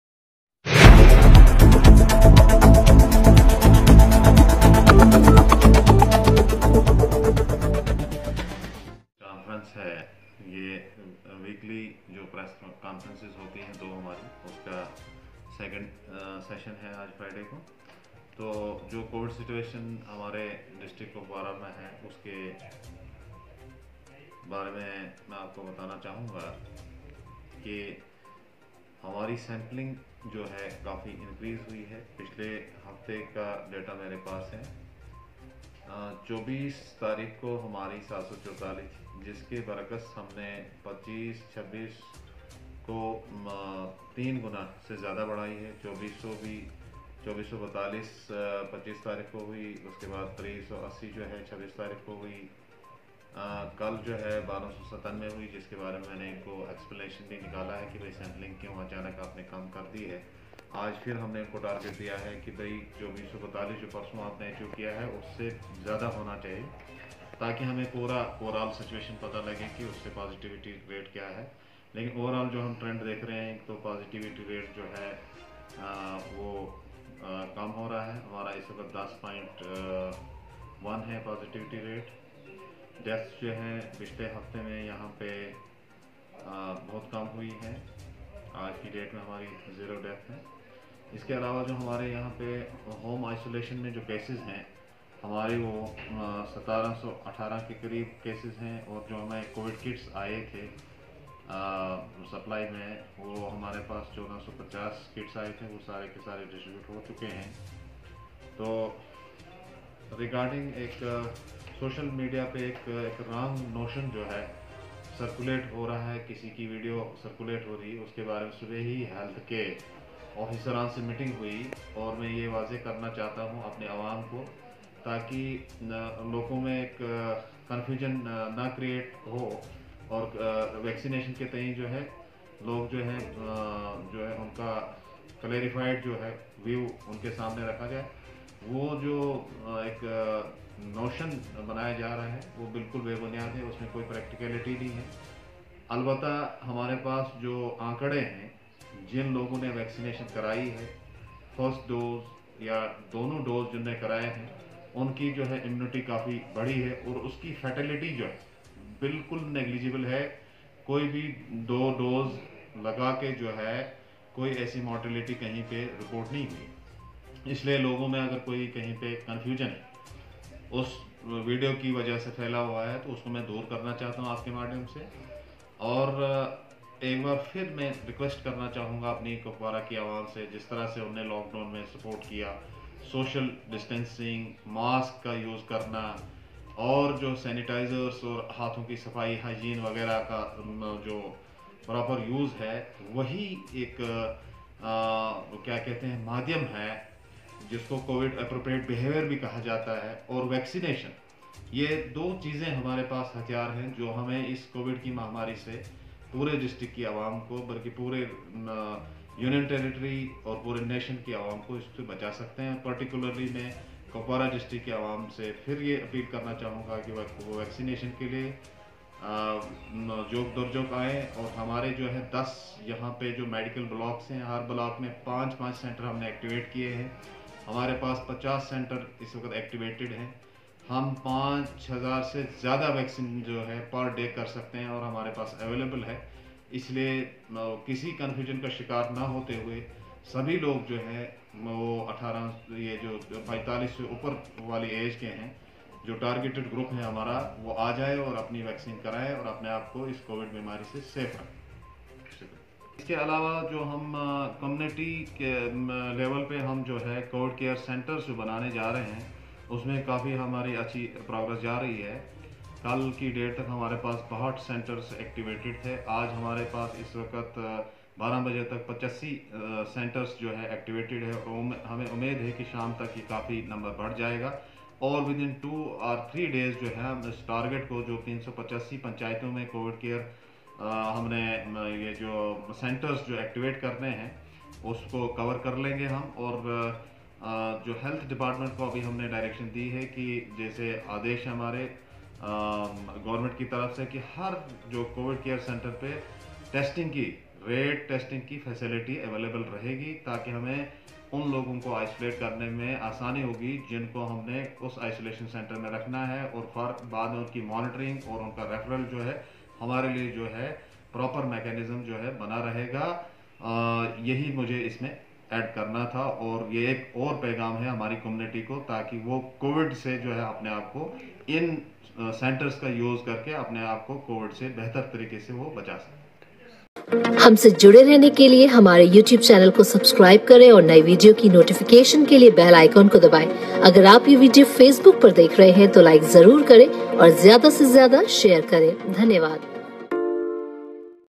Sampling intensified across district to contain the pandemic: DC Kupwara | Kupwara Times
Sampling intensified across district to contain the pandemic: DC Kupwara